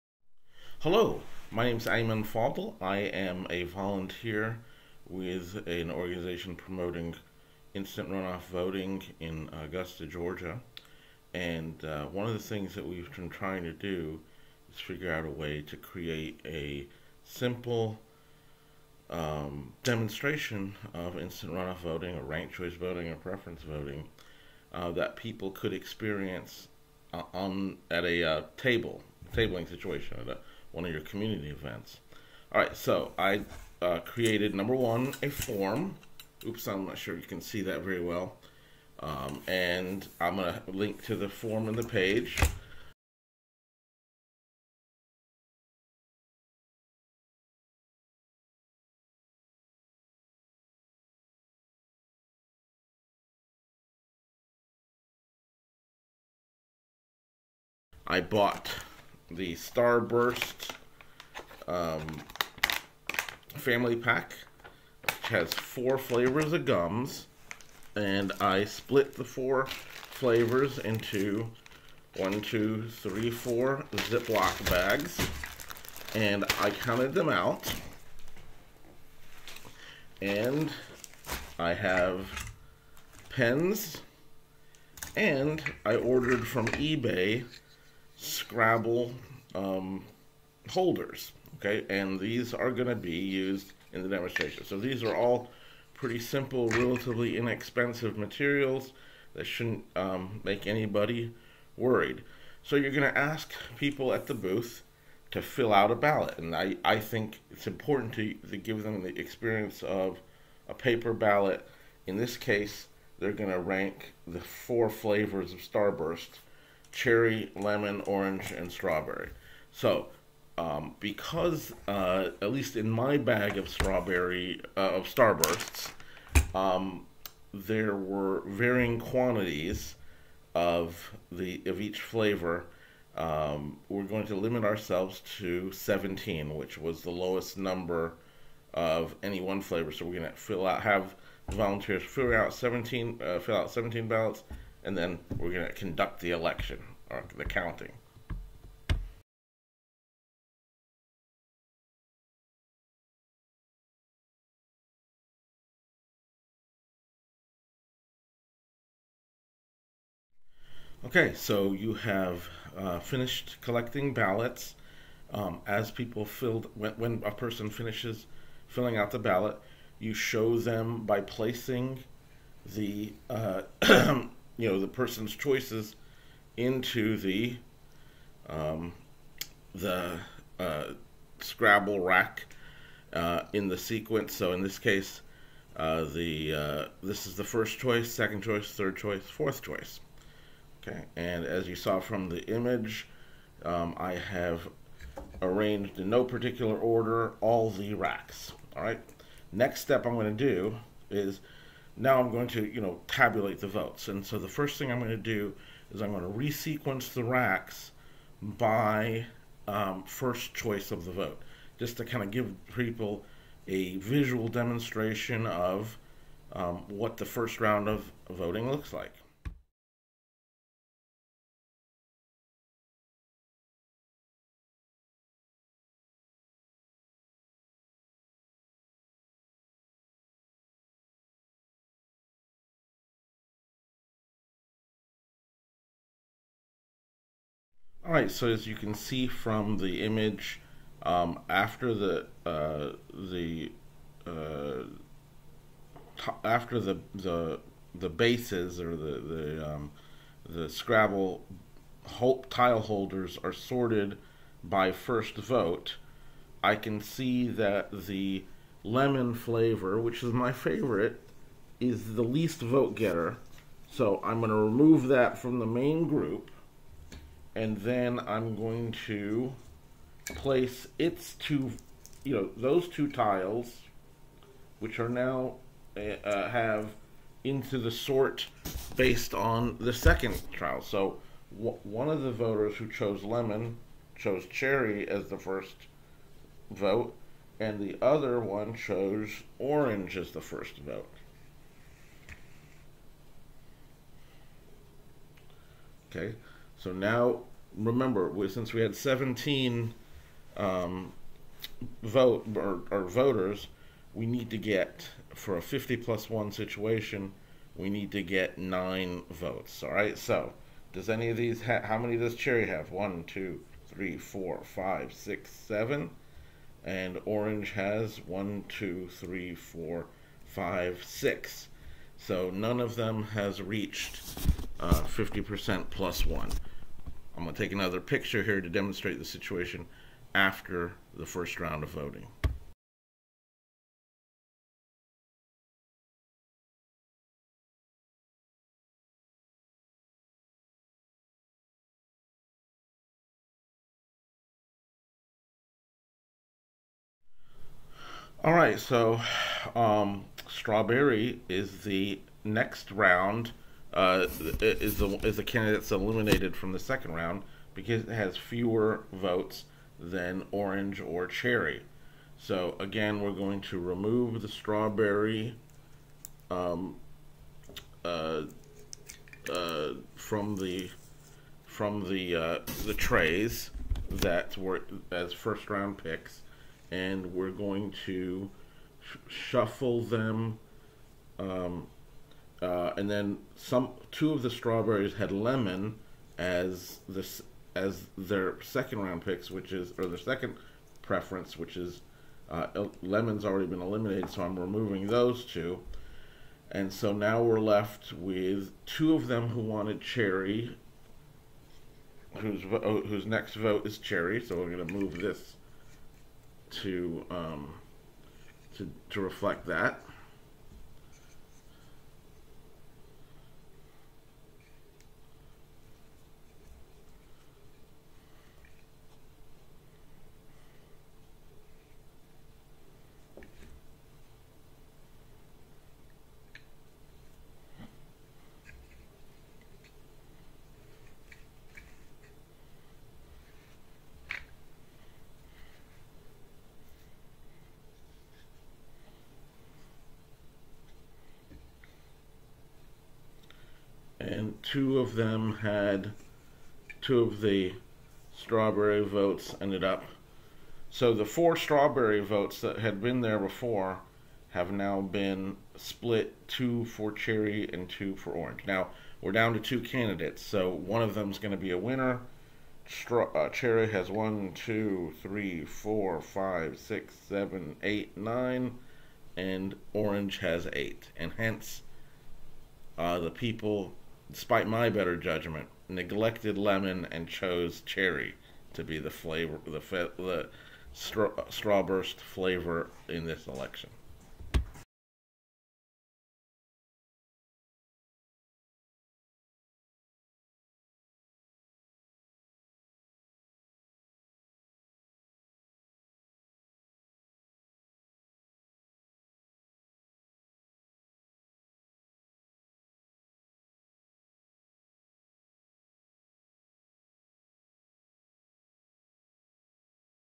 How to Do an Instant Runoff Voting Demonstration at a Tabling Event
Advocates for instant runoff/ranked choice/preference voting can use simple, inexpensive materials to demonstrate to people how this method of voting works.

eBay search for "scrabble tile holders"